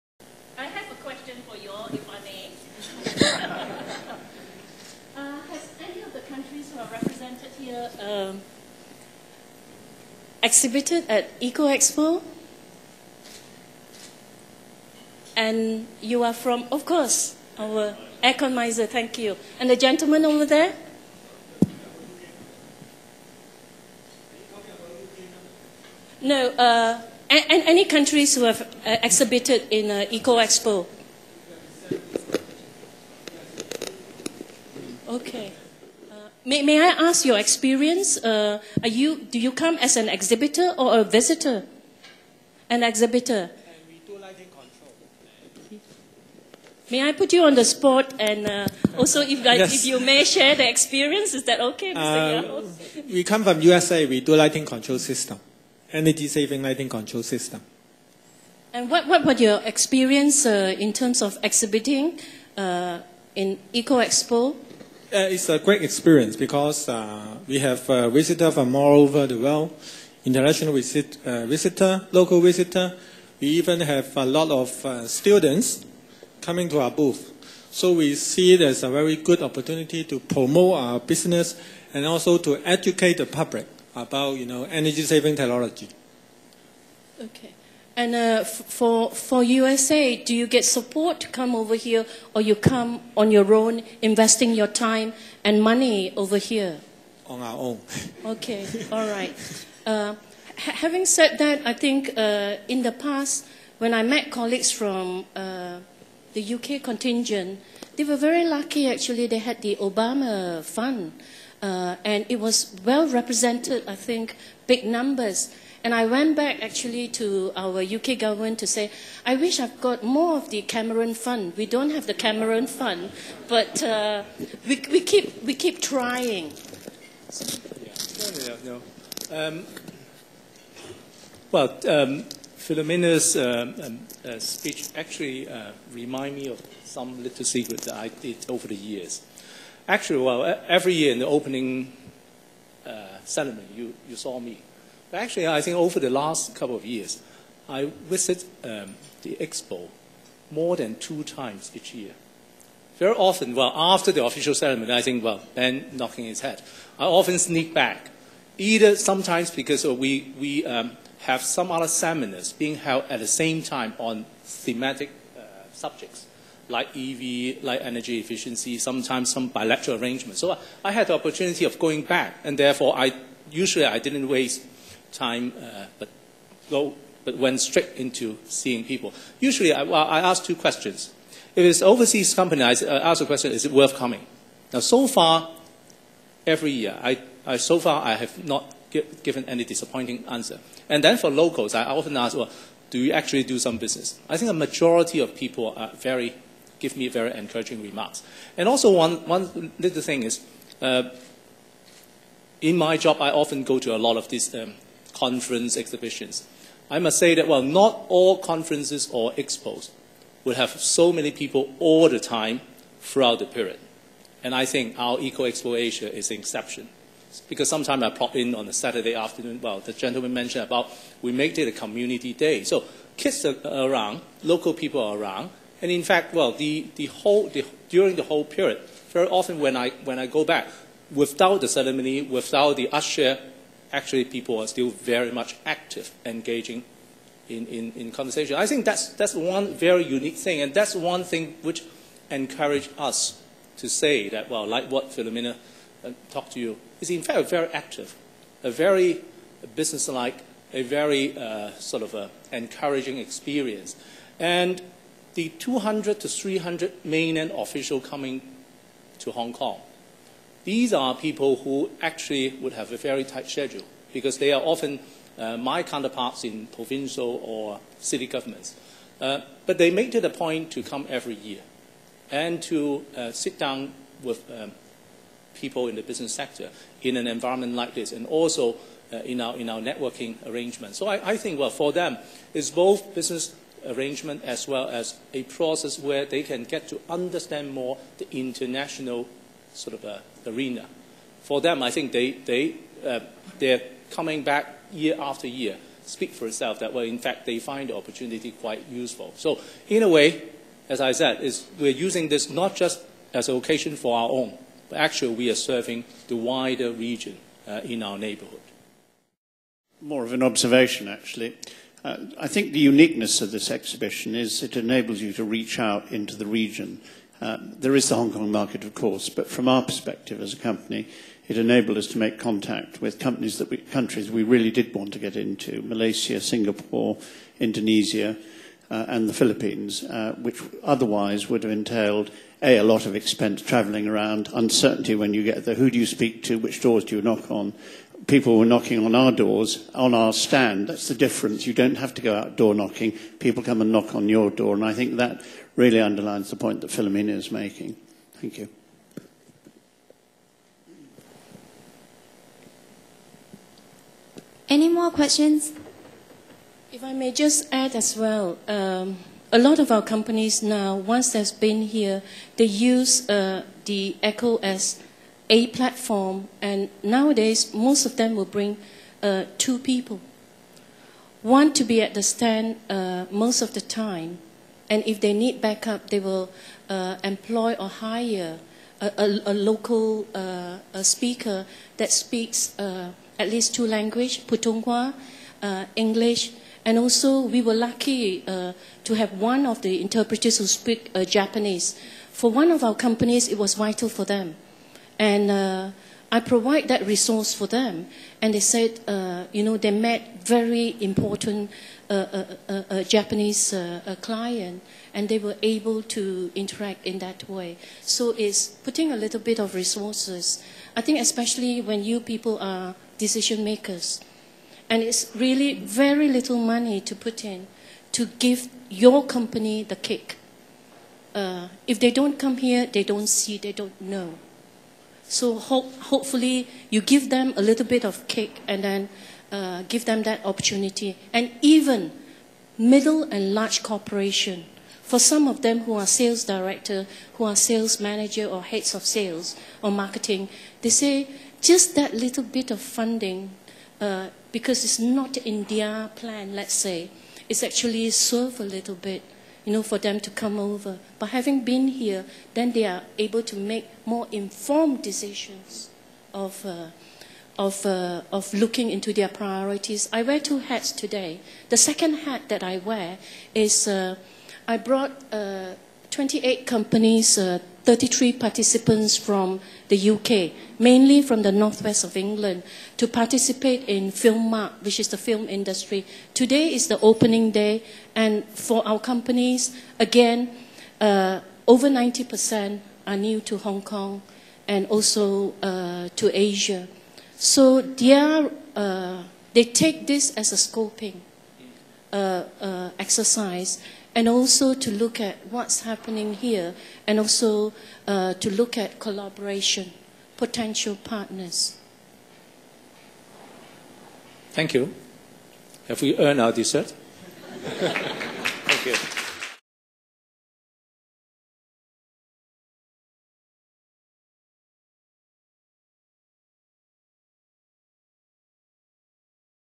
Eco Expo Asia: Q & A on Business Opportunities in Sustainable Development
Bringing forward hot topics from the Eco Expo Asia, speakers are sharing with audience at the Networking Luncheon on "Business Opportunities in Sustainable Development."